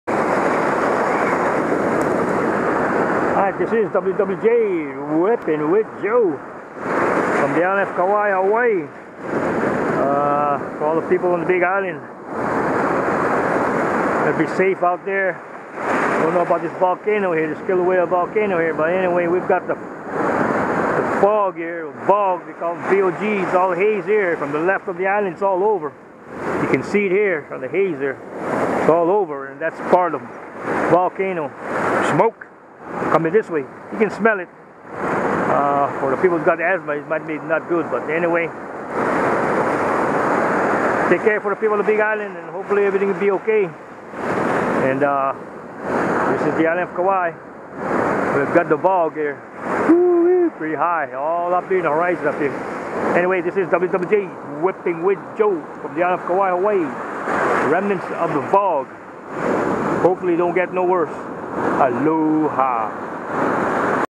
WWJ Kilauea Volcano VOG on Kauai
WWJ whipping with joe..caught a good sized toau..but forgot to turn on the video..then seen the VOG..you can smell it first before I noticed the haze..